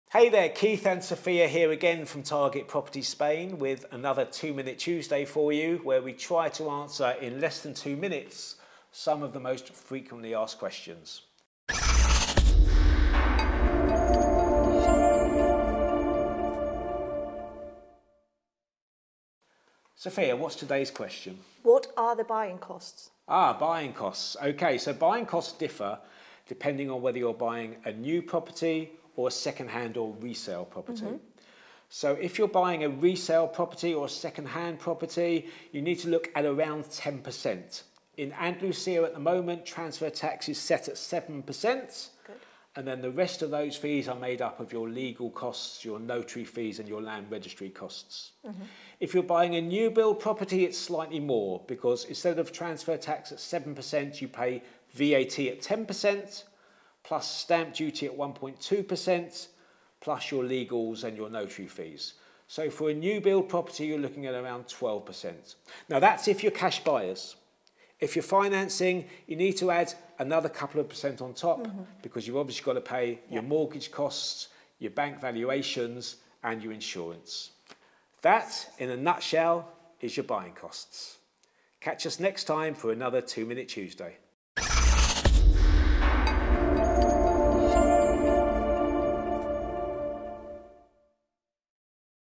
What are my buying costs in Mijas Costa? | 2 Minute Tuesdays Ep4
🏡 Welcome back to 2 Minute Tuesday! In Episode 4, Keith and Sophia from Target Property Spain are here to answer one of the most asked questions: "What are my buying costs for a Mijas Costa property?" 🌴

Curious about the expenses involved in purchasing your dream property in Mijas Costa? Keith and Sophia break it down for you in under 2 minutes, providing valuable insights into the various costs you need to consider as a buyer.

🚀 In this episode, you'll discover:

The different buying costs involved in acquiring a property in Mijas Costa, including taxes, notary fees, and more.
Tips on how to budget effectively and avoid any surprises during the buying process.
Whether you're a first-time buyer or looking to invest in the Mijas Costa area, this episode is packed with practical advice to guide you through the purchasing journey. 🌟

🛎️ Take the first step towards your dream property in Mijas Costa by tuning in now with Keith and Sophia! 🌐✨